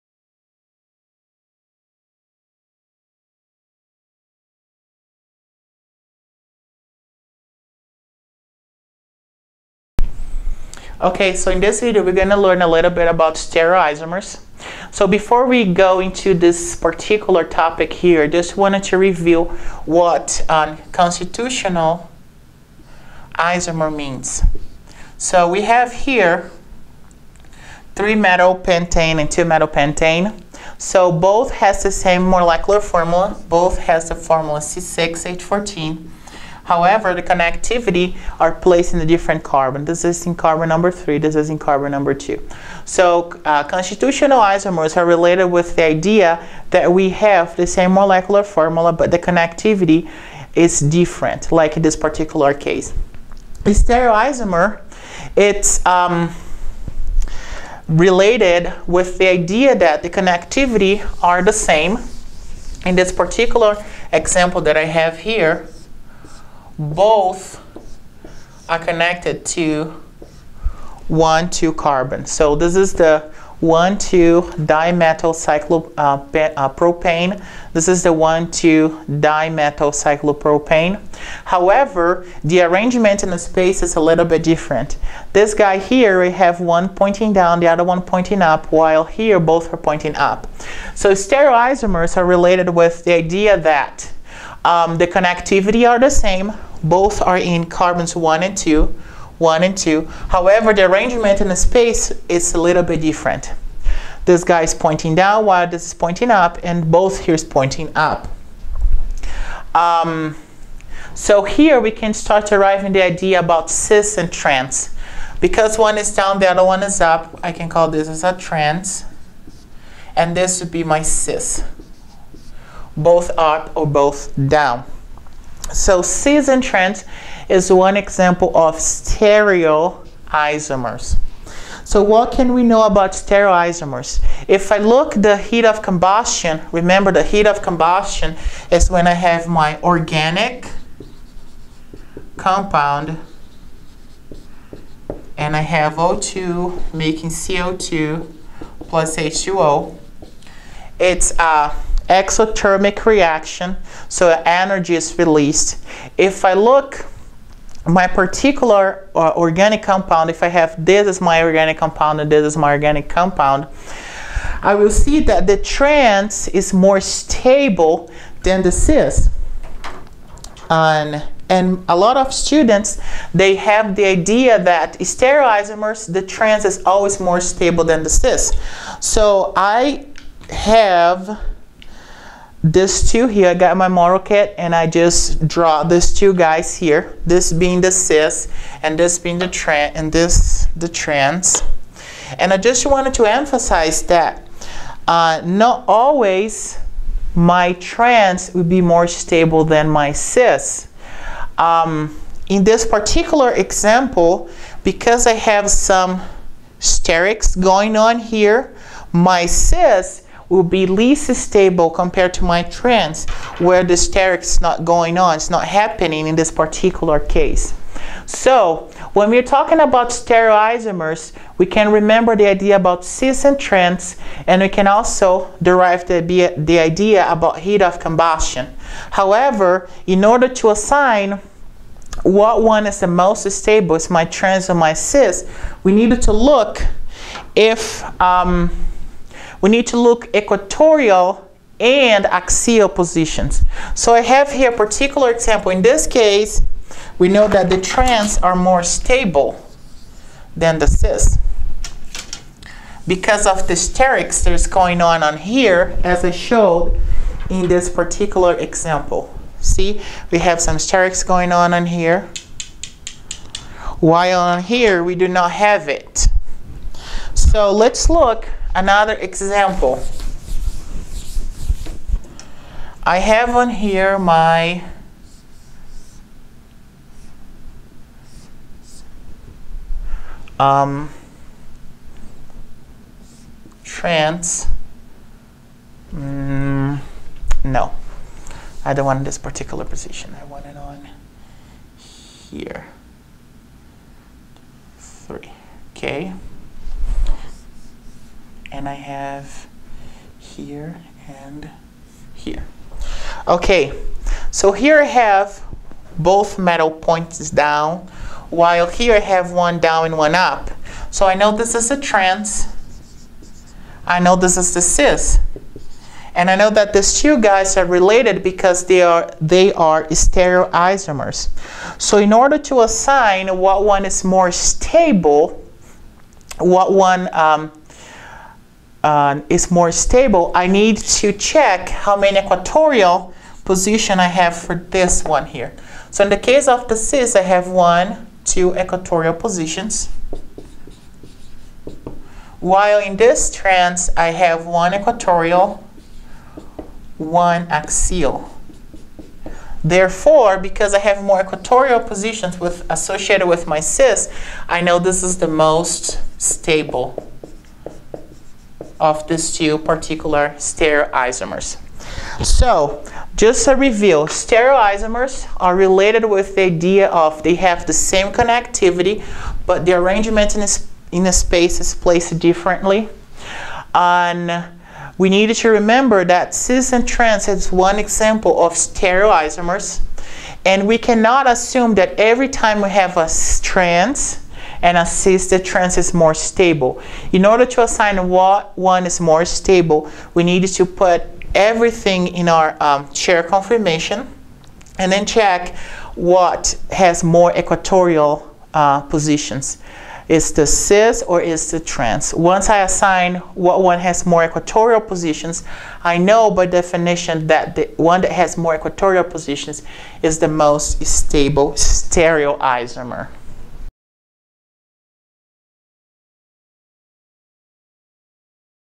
Stability of Cis and Trans Stereoisomers
This Organic Chemistry video discusses the stability of cis and trans stereoisomers